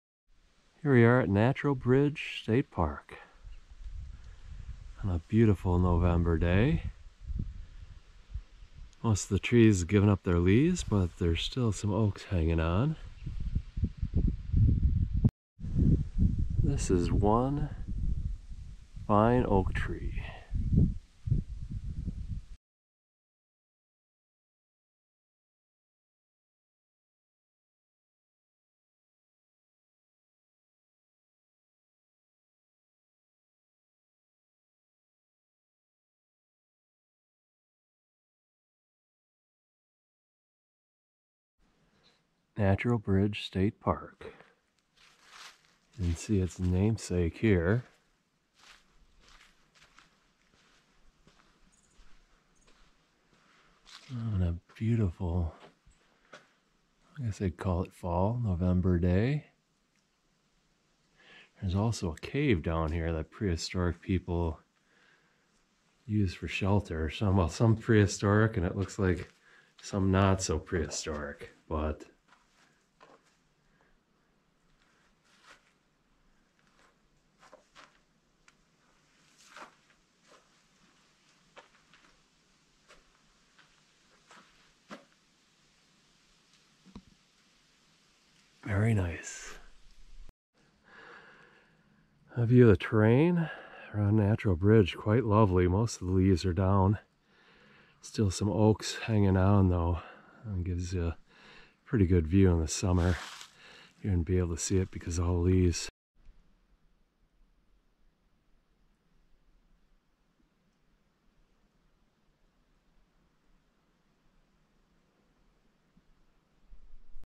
Wisconsin State Parks and Natural Areas - Natural Bridge State Park
Natural Bridge State Park is one of the smaller, and lesser known parks in the Wisconsin State Park System. Take a short tour of this scenic and unique park with its namesake bridge.